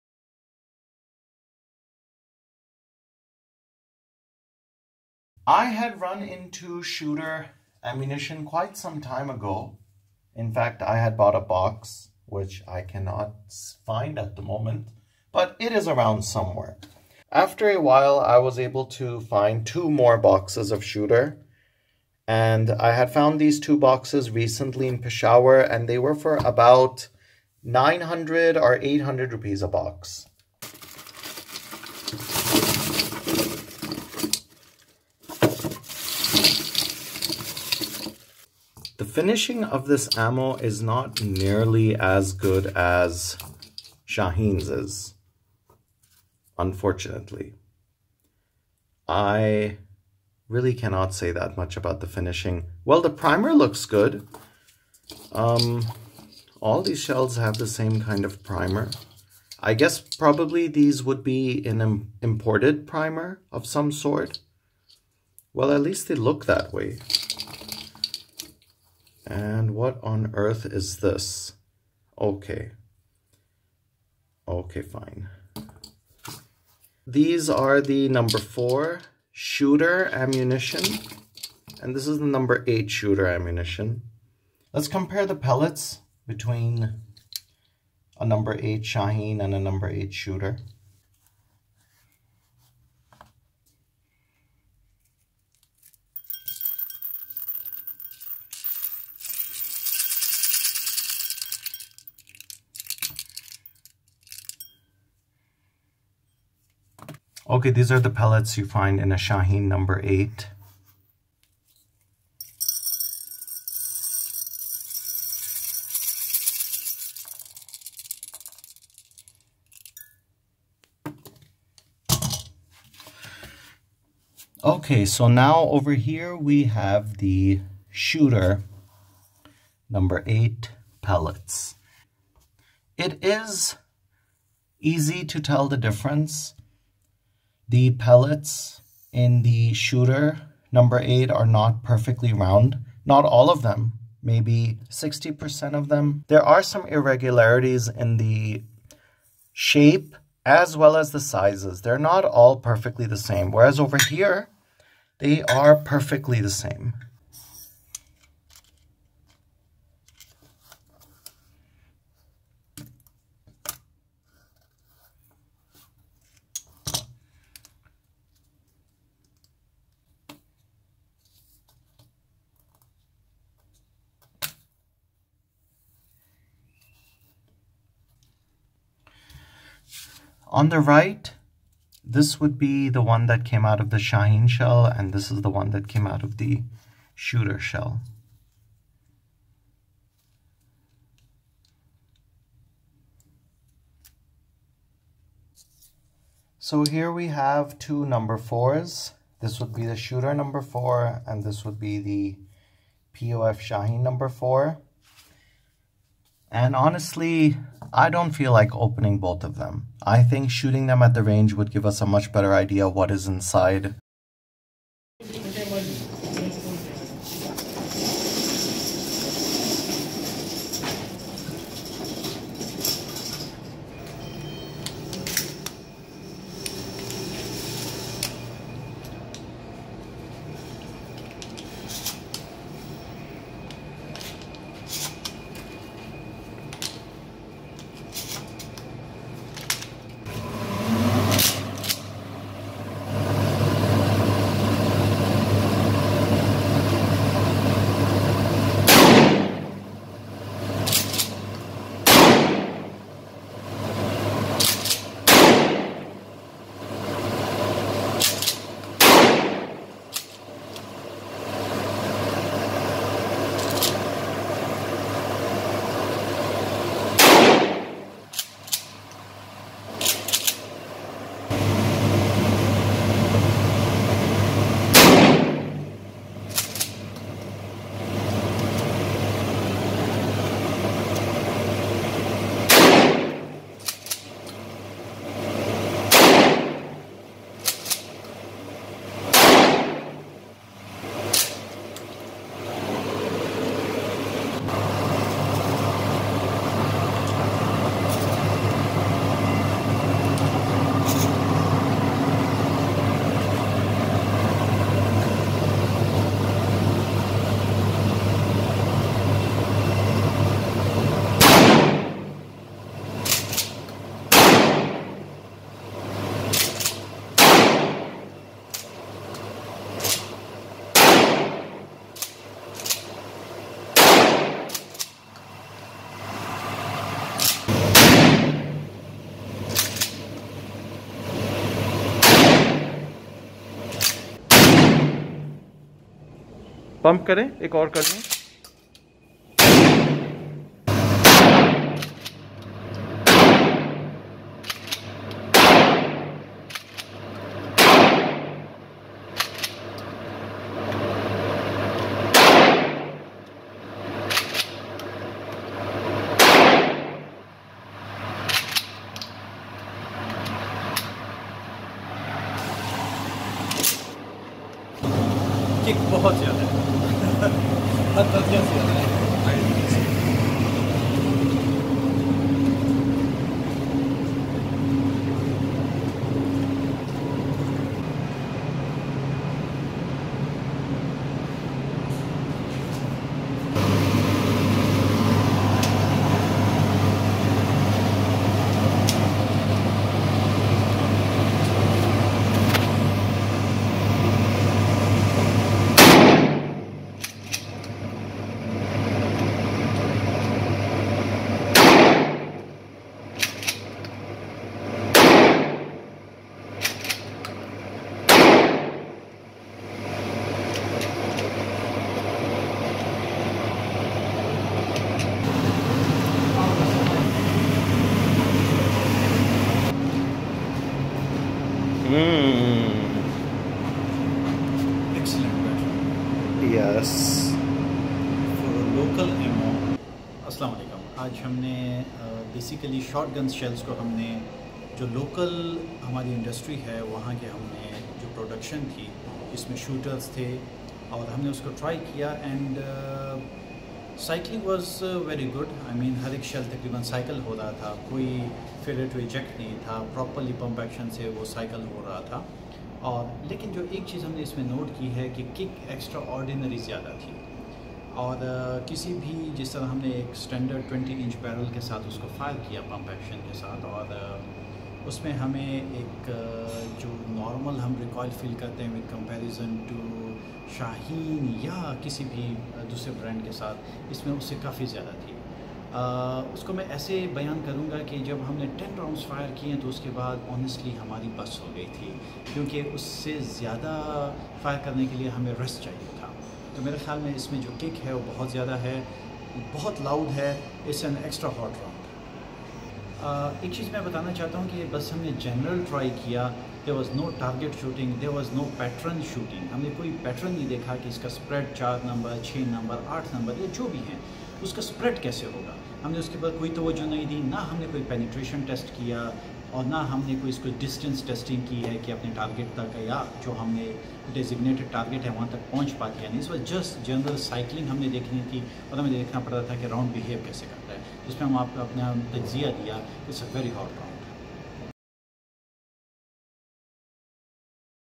Shooter Shotgun Cartridges
With the ever increasing price  of POF`s shotgun shells the need to find an alternate source of shotgun shells has never been greater.
Shooter is infact an old brand..it was once known as SUNSHINE.
I had bought a box of shooter shotgun shells about 5 or 6 years ago but never fired a round of it..i dont hunt alot and when i do i prefer quality over quantity so never got around to using it.
With prices of POF`s shotgun shells going insane,Shooter shells began appearing on the market everywhere...and i decided to get two boxes to test and try out.

The general finishing of the shells wasnt great and the mat i emptied the boxes on even got quite dirty.
Looking at the general finishing of the Shooter shells did however make me appreciate the finishing of the POF Shaheen shotgun shells.
Surprisingly the shells worked and cycled quite well...no misfires or any malfunctions of any sort happened. however...the recoil of the shells was massive.
The recoil was massive and violent and POF`s shells felt as if they had no recoil compared to the shooter shells.
The Shooter shells gave off alot of smoke.
Maybe the Shooter shells deserve a good pattern test to see how accurate they are. but judging by the poor shape of the pellets i highly doubt it would be a fruitful test.
The utility of such a cartridge is obvious...it is meant to fodder for those who can no longer afford Shaheen POF ammo.
Due to the smoke and massive recoil,I wouldnt recommend using this shell in an expensive firearm.